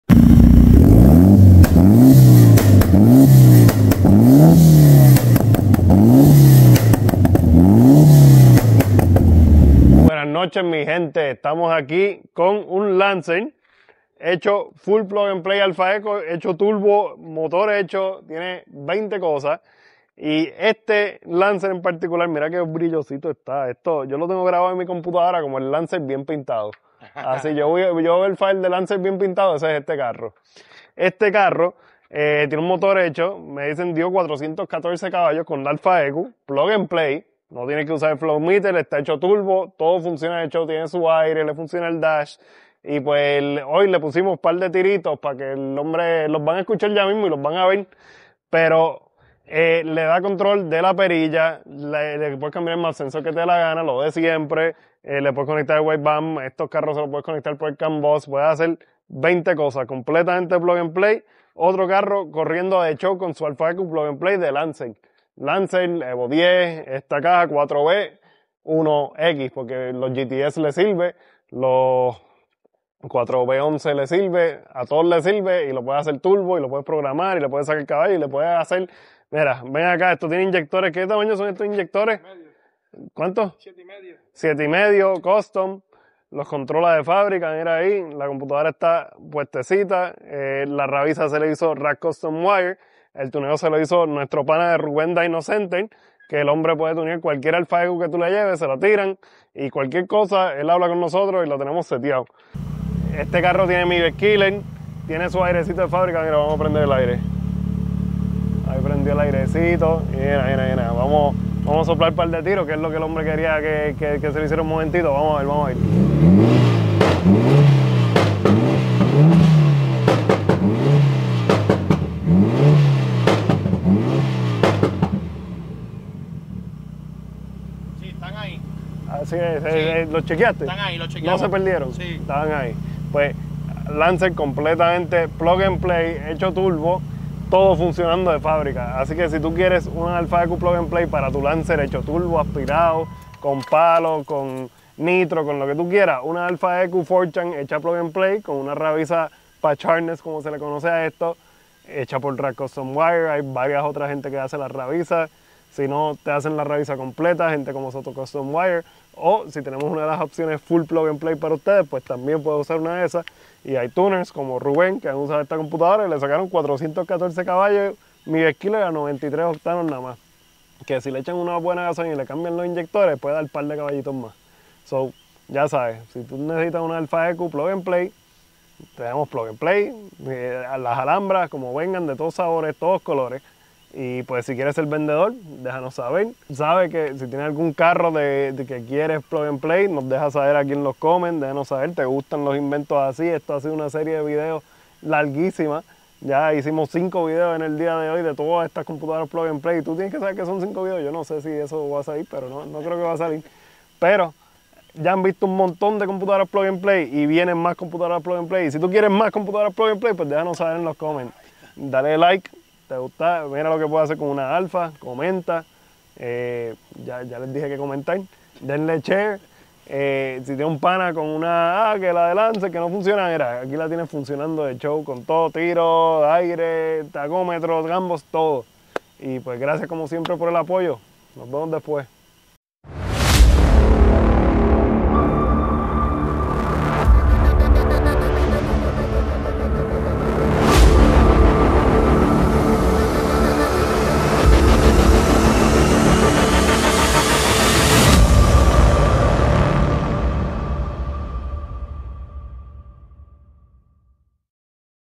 Un 4B11 con una PLUG AND PLAY corriendo de show! Mini review
La LancerPNP esta probada! Consigue la tuya ya!!

Tenemos cinco libros sobre los temas de las ECU originales y aftermarket! Conseguiste los tuyos ya?